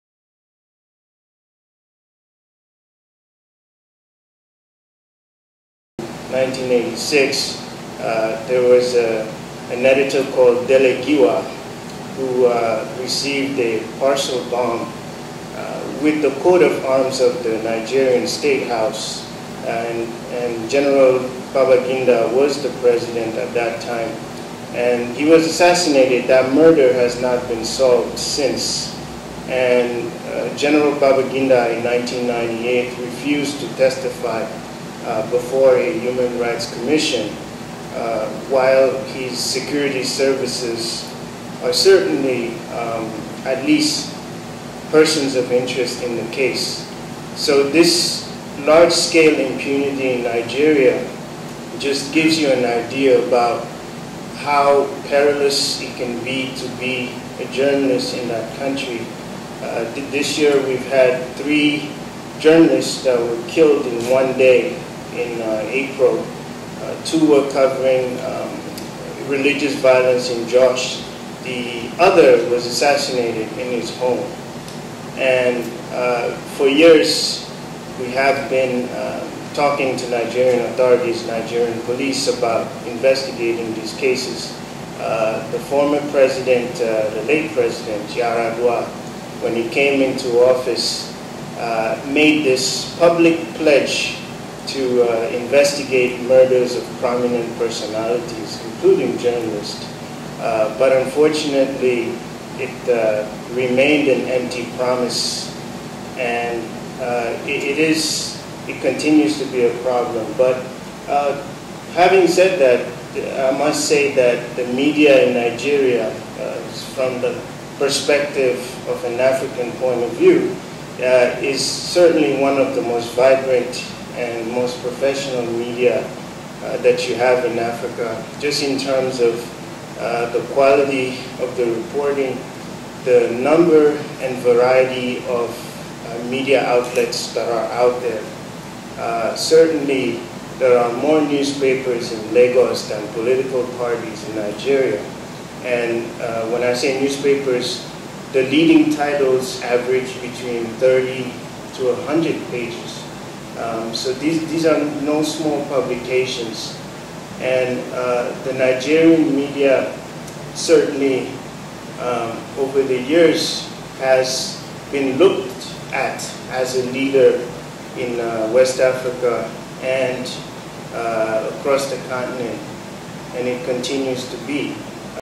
CPJ about Ibrahim Babangida
The Africa Advocacy Coordinator for the Committee that Protects Journalists (CPJ), speaks out about IBB, Ibrahim Badamasi Babangida in regards to the death of the late editor, Dele Giwa.